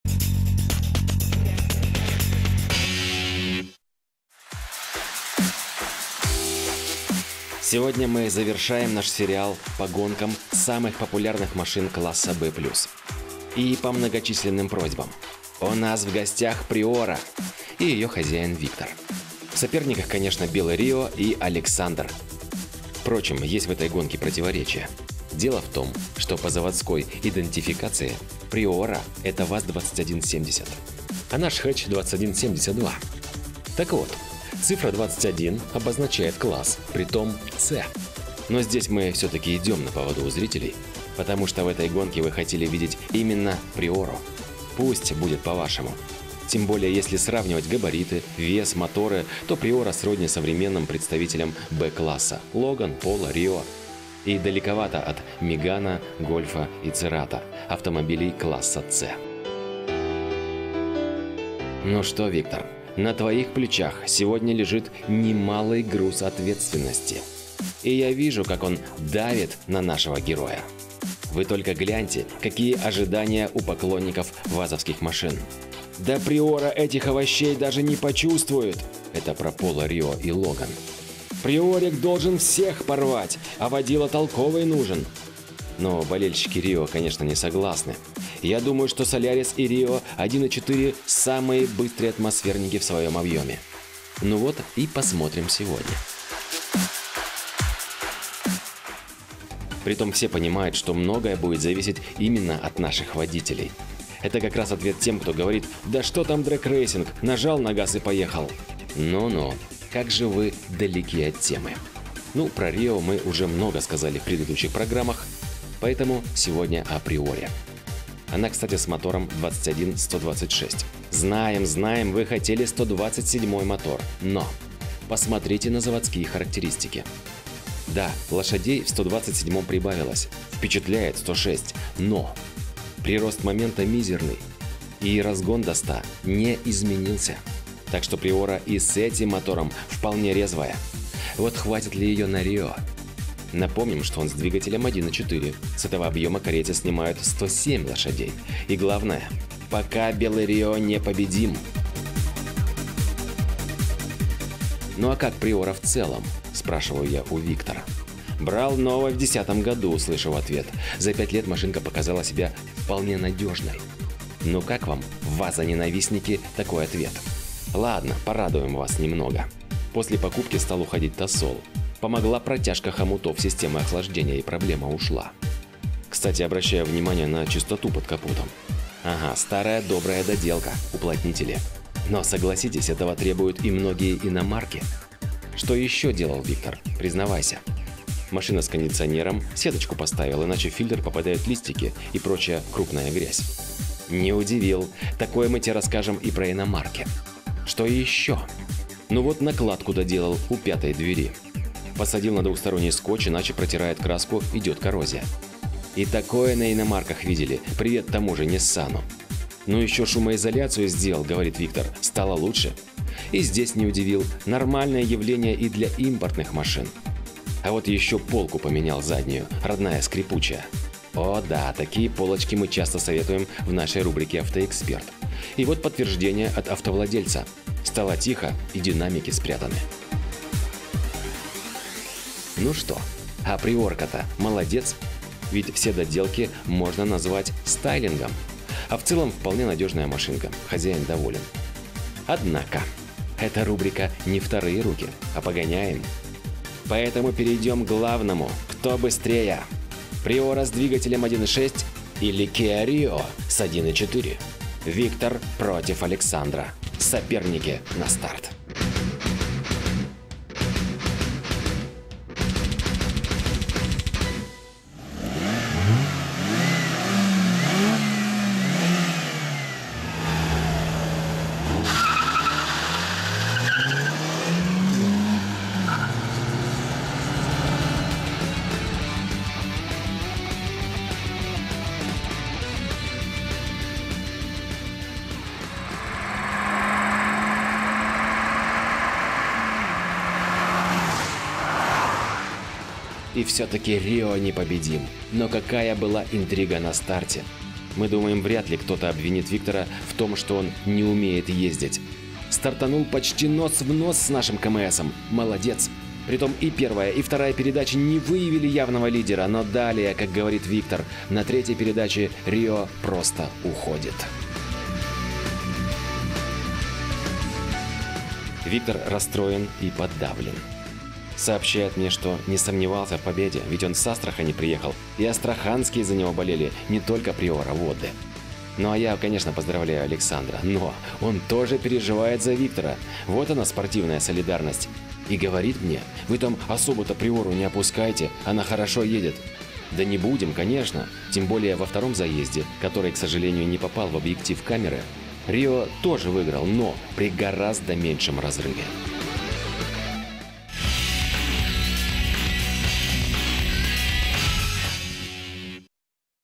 KIA Rio 1,4 vs Приора 1,6
Любительский дрэг. Обгонит ли KIA Rio 1,4МТ Ладу Приору 1,6МТ?